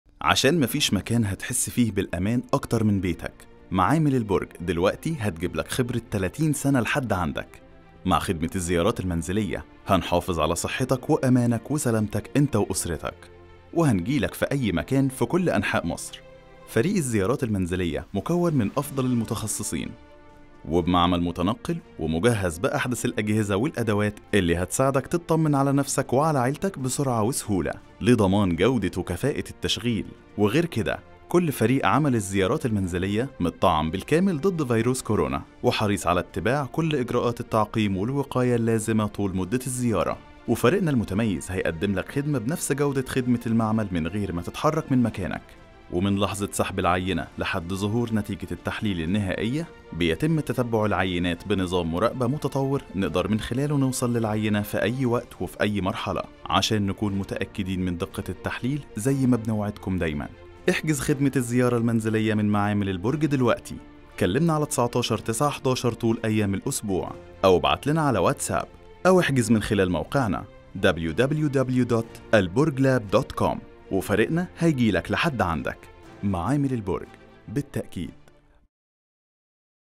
٣٠ سنة حواليك ومعاك - خدمة الزيارة المنزلية - معامل البرج
مش قادر تنزل في الزحمة عشان تحلل؟ احنا هنجيلك لحد عندك
معامل البرج بقالها٣٠  سنة حواليك ومعاك وعارفين بيوت مصر كلها فتقدر دلوقتي تحجز بكل سهولة الزيارة المنزلية وهيوصلك أكفأ فريق متخصص لسحب العينات في البيت بأحدث الأجهزة والامكانيات وهتستلم نتيجتك على موبايلك.
دلوقتي تقدر تدفع كل تحاليلك من خلال طرق مختلفة زي اونلاين أو كاش أو فيزا.
وعشان سلامتك بنطمنك إن كل أخصائيين الزيارة المنزلية مطعمين ضد فيروس كورونا.
كلمنا وهتلاقينا دايمًا حواليك ومعاك انت وأسرتك.
رقم الخط الساخن:
19911